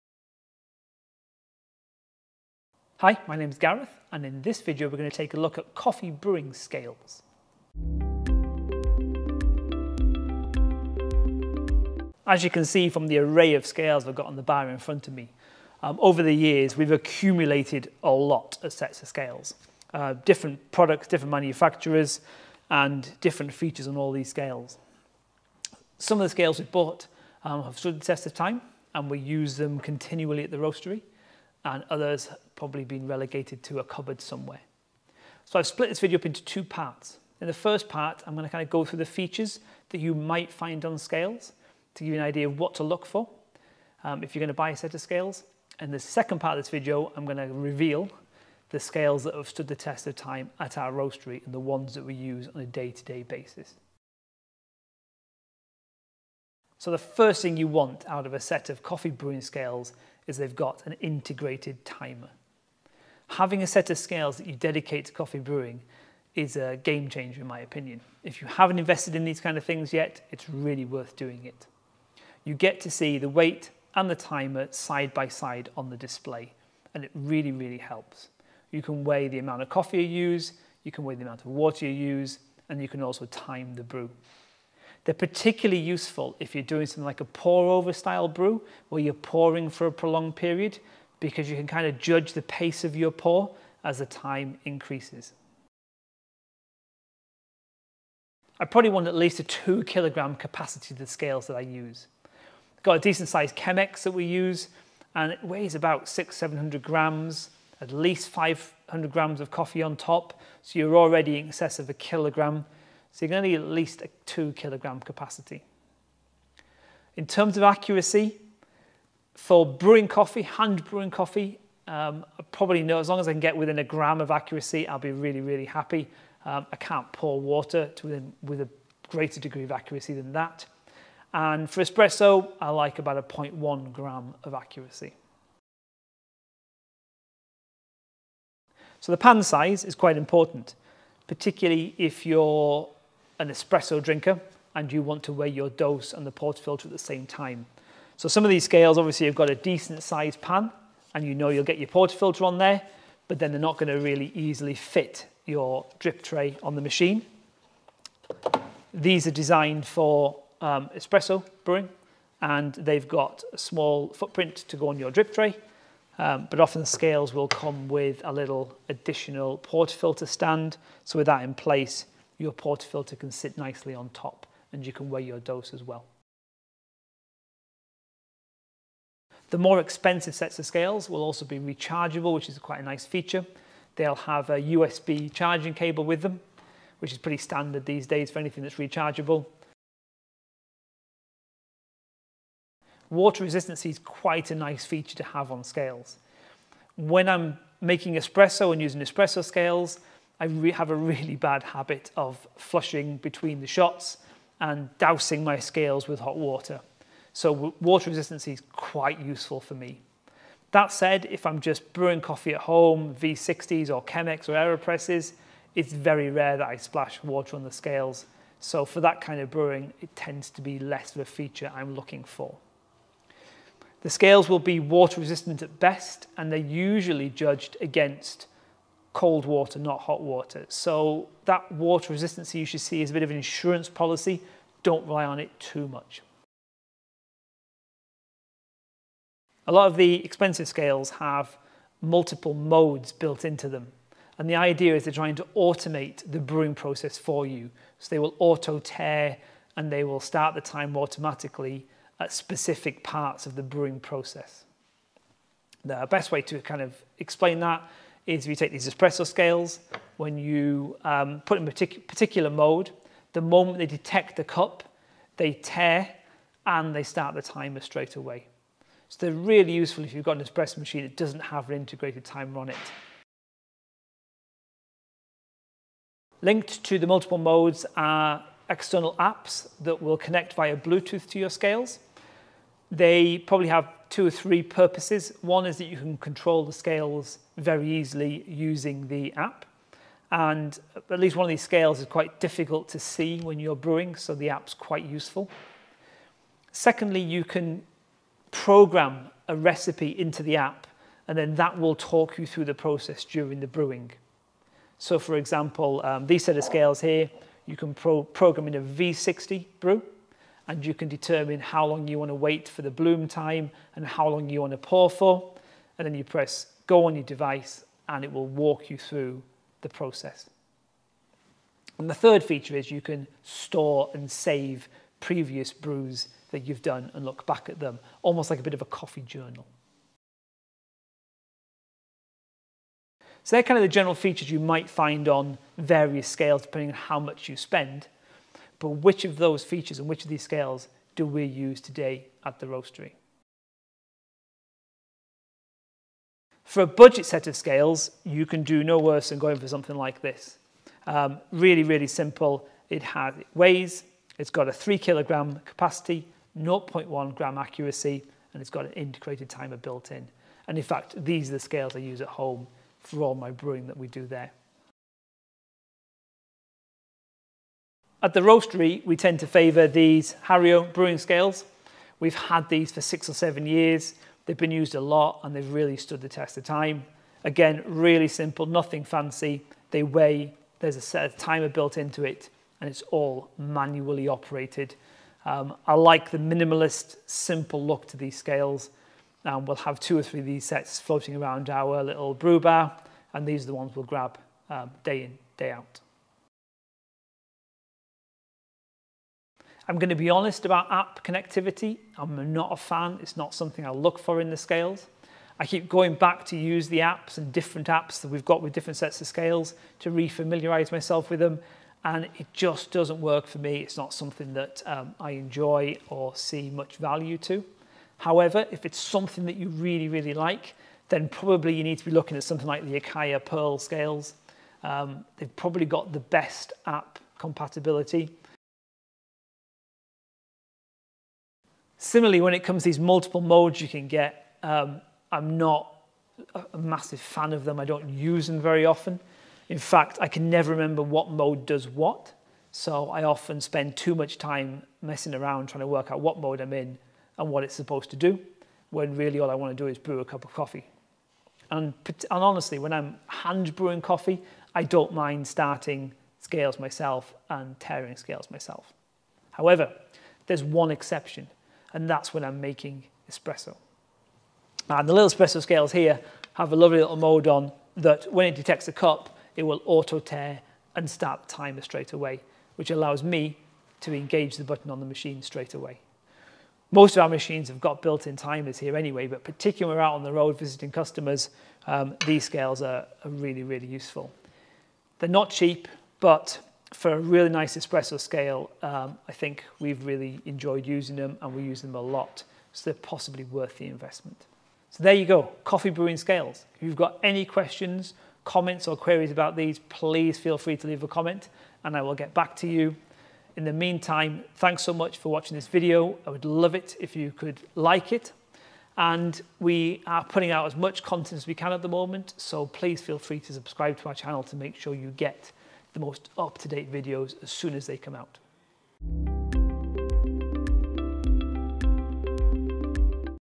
Coffee Brewing Scales
A set of scales are one of the key pieces of kit to invest in for brewing coffee at home ⚖️

In this video we look at the features you can expect to find on the various coffee brewing scales which are available on the market.

We also reveal the scales which we use most at the Roastery, the ones which have stood the test of time!

05:30 - Jump straight to the reveal of our favourite scales

Comments, queries and questions are welcomed.